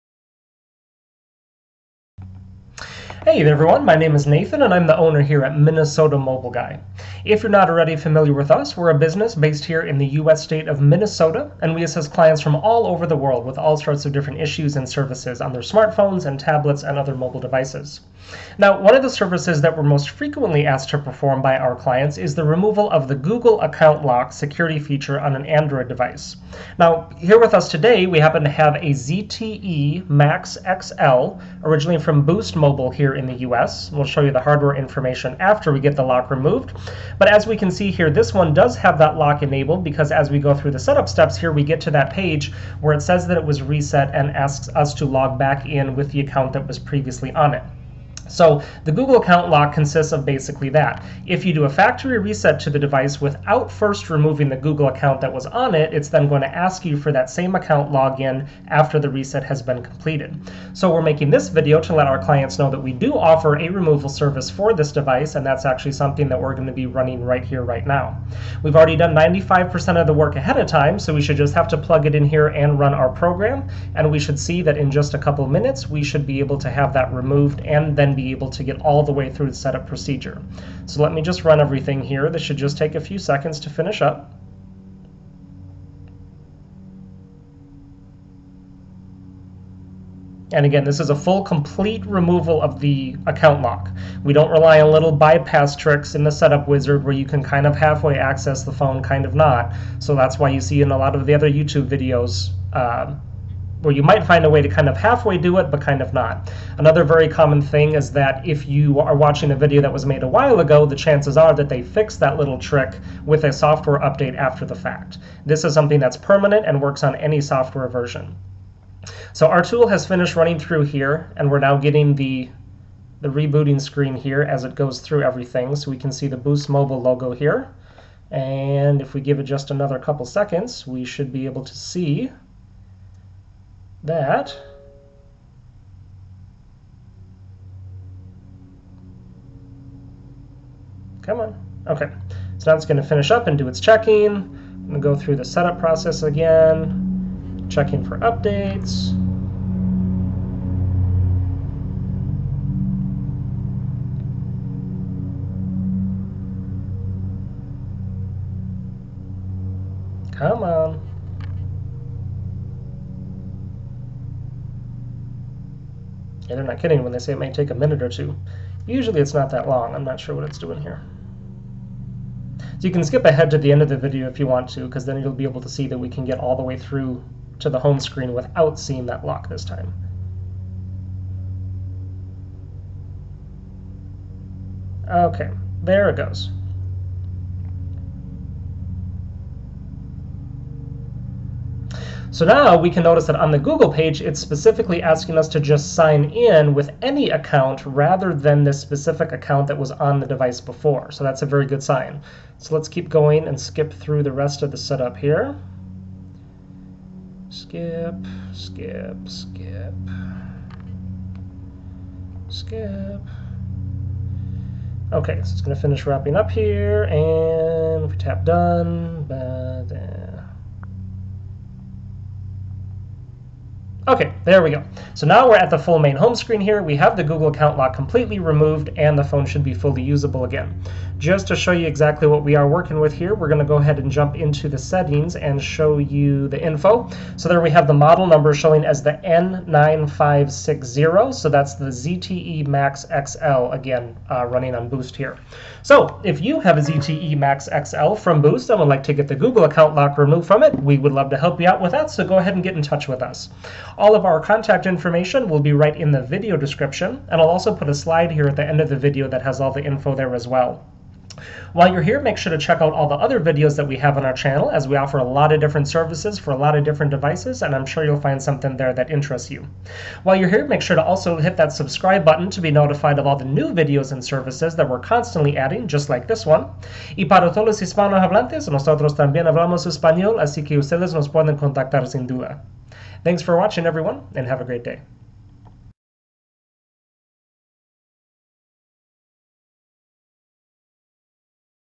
Disable Bypass Remove Google Account Lock FRP on Boost Mobile ZTE Max XL!
Video showing a Google Account Lock successfully being bypassed on a Boost Mobile ZTE Max XL (N9560). Google includes this security feature with all phones on Android 5.1 and up so that when a device is reset without first removing the Google account, the device is locked to the previous owner's account. This process can be done to the ZTE Max ZL by allowing a bypass of the screen that asks for the Google account of the previous user.